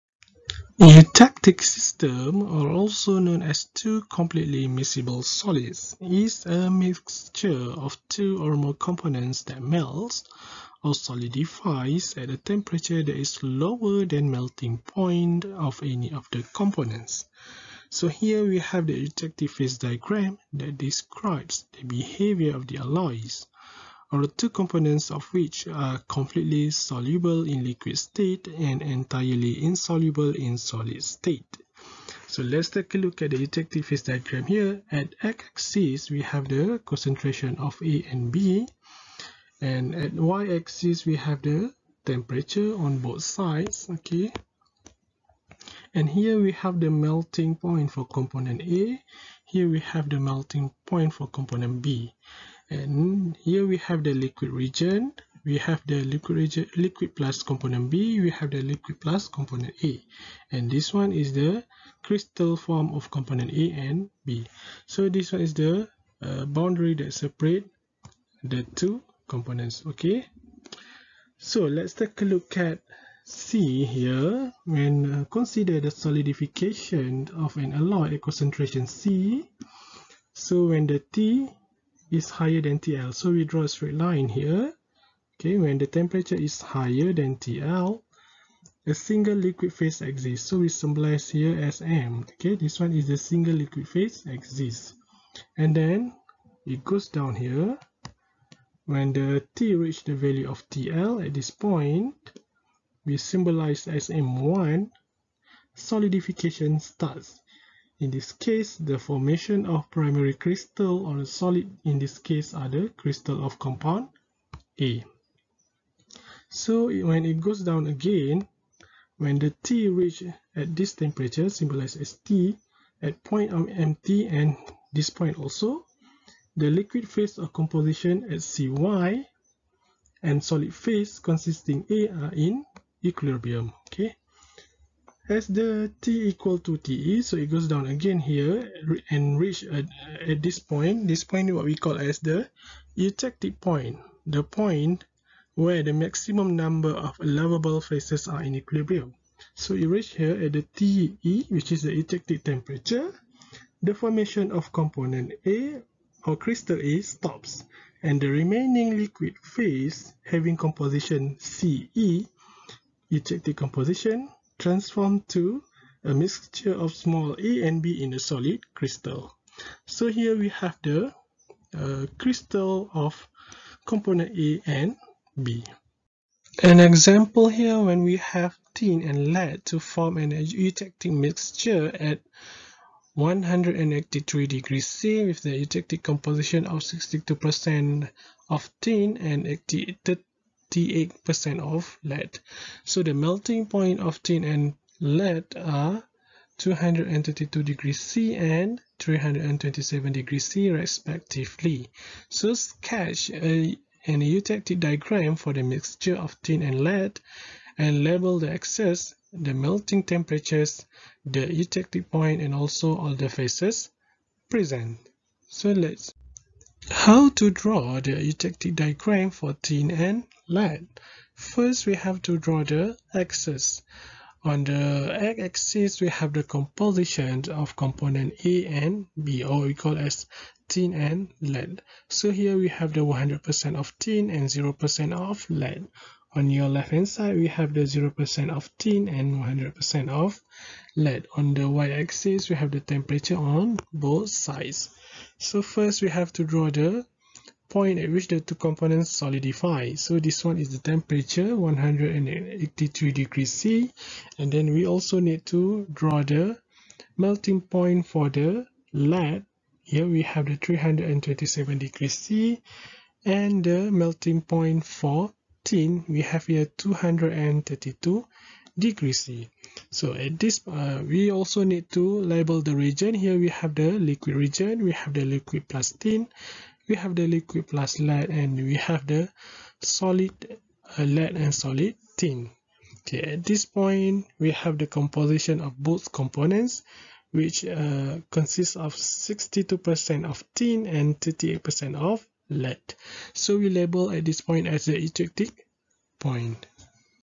Eutectic system of two miscible solids
Understanding simple eutectic system with two completely miscible solids